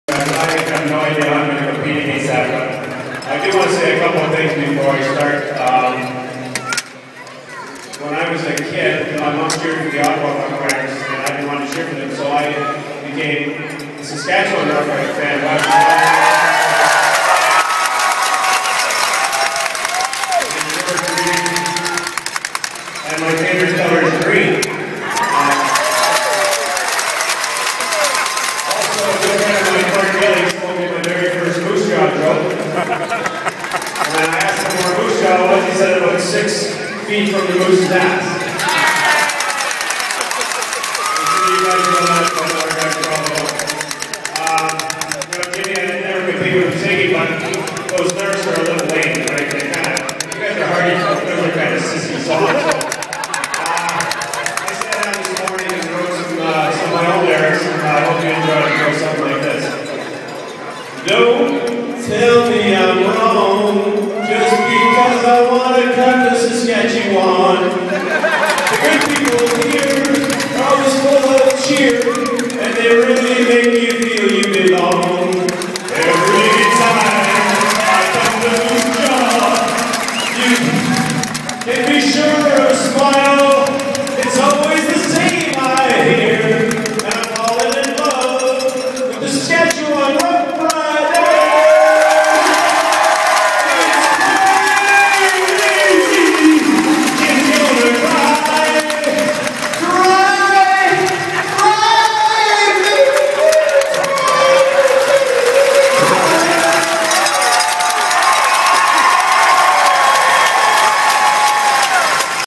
Mark Napier JUNO Cup Sing Off 2013
Mark Napier "Tries" to outperform Jim Cuddy